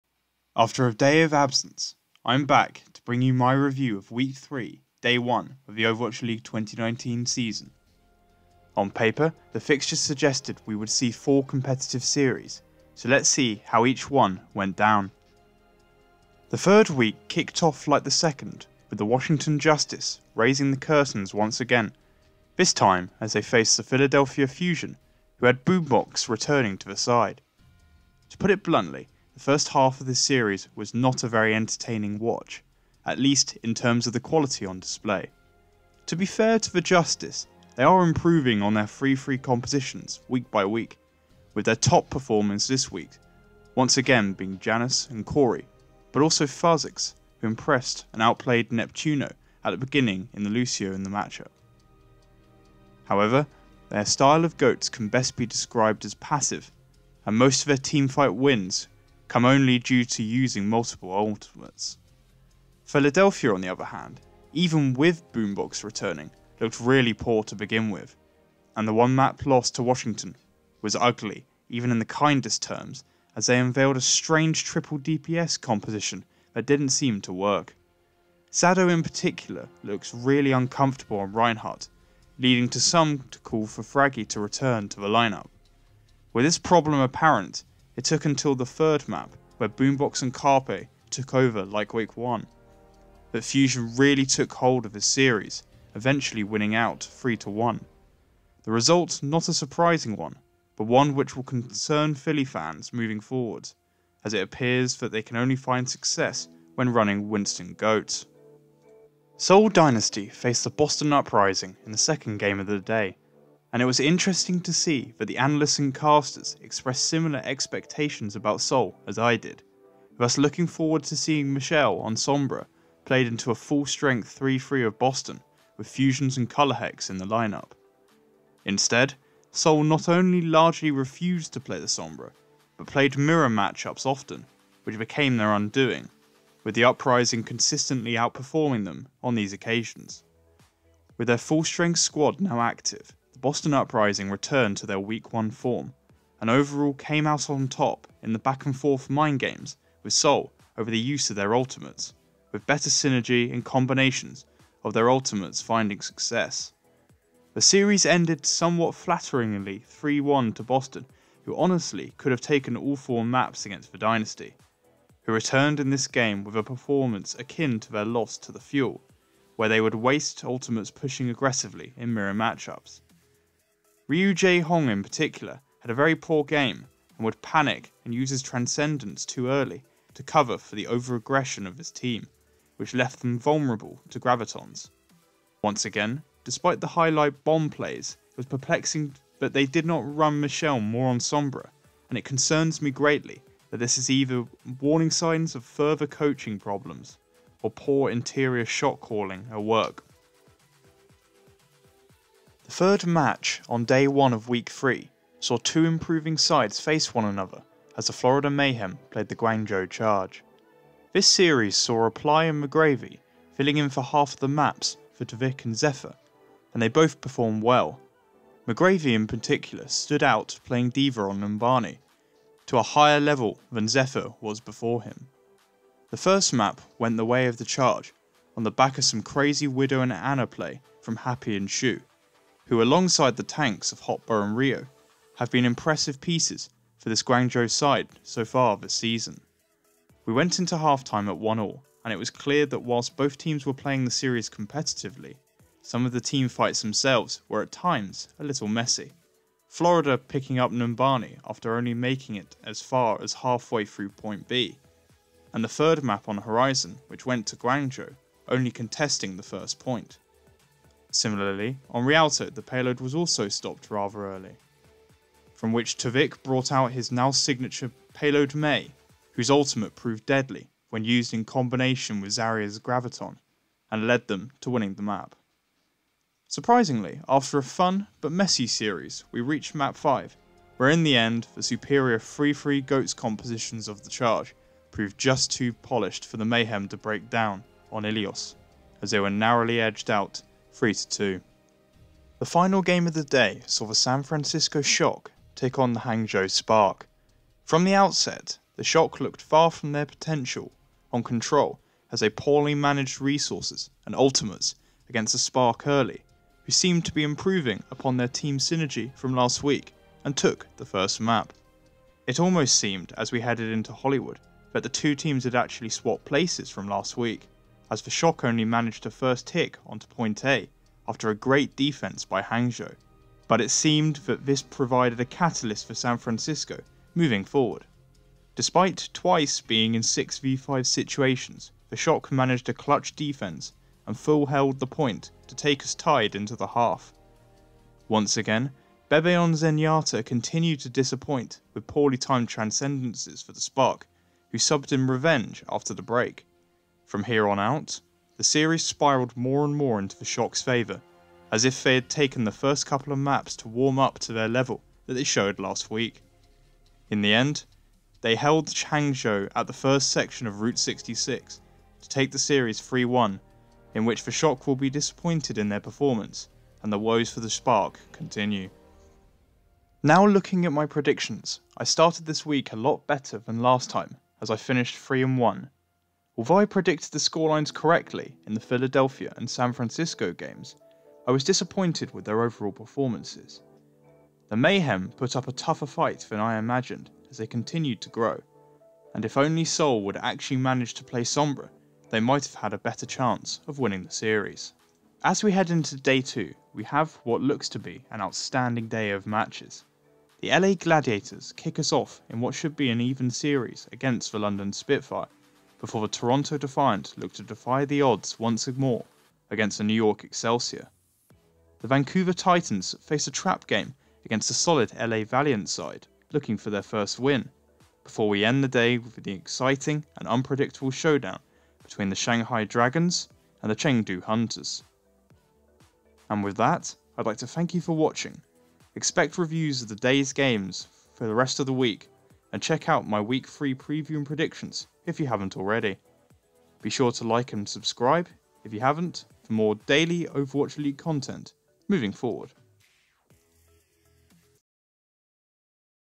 Overwatch League Week 3 Day 1 Review
This is my review for the first day of the third week of the Overwatch League 2019 season (Stage 1, Week 3, Day 1) where I discuss the four games and look back at the week to see how my predictions fared.